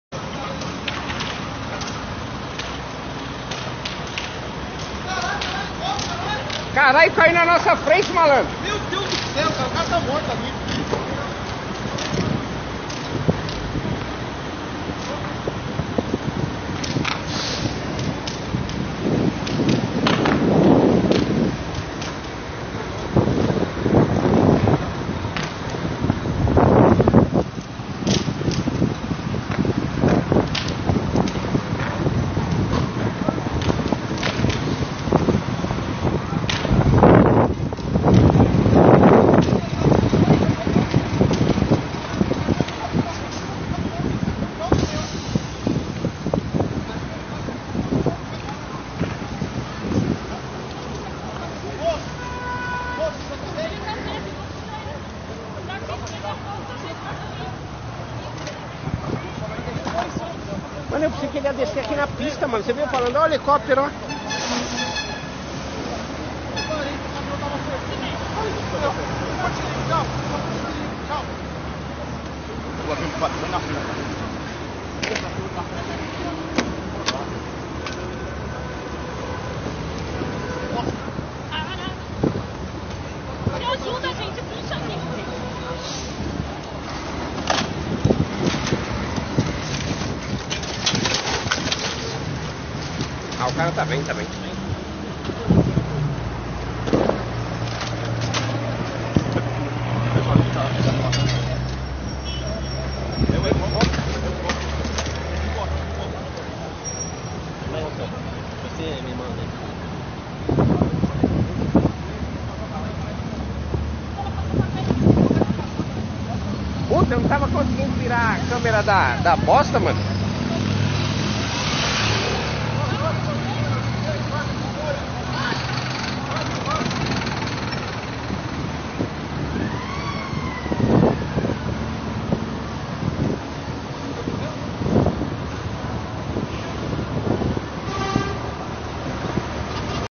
MOMENTO DA QUEDA DO HELICÓPTERO QUE MATOU RICARDO BOECHAT, JORNALISTA DA BAND
A queda de um helicóptero nesta segunda-feira (11), no Rodoanel, em São Paulo, matou o jornalista Ricardo Boechat. A aeronave ainda bateu na parte dianteira de um caminhão que transitava pela via.

De acordo com o Corpo de Bombeiros, ele e o piloto morreram carbonizados. Houve uma terceira vítima com ferimentos.

A queda ocorreu no quilômetro 7 do Rodoanel, sentido Castelo Branco, perto do acesso à Rodovia Anhanguera.